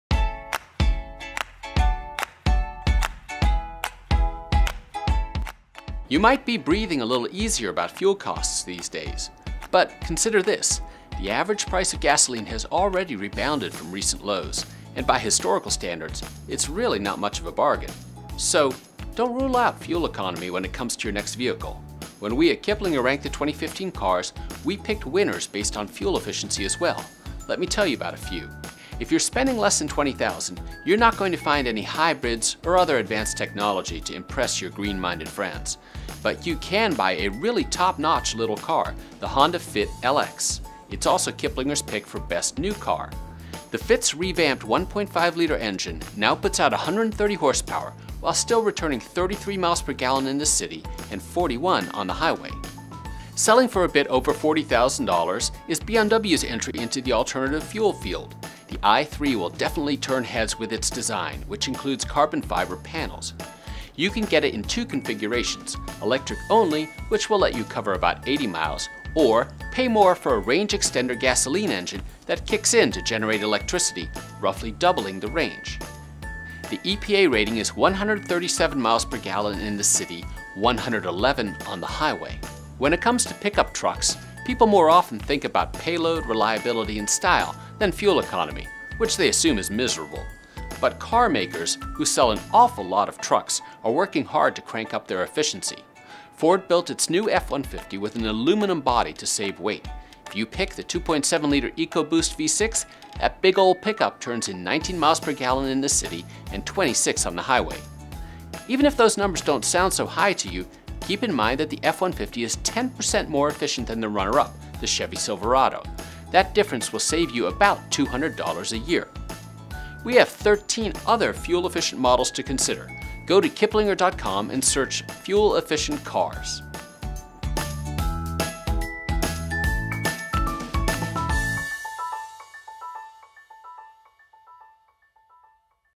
Most Fuel-Efficient Cars of 2015
Three affordable new vehicles that will help you get more mileage for your money.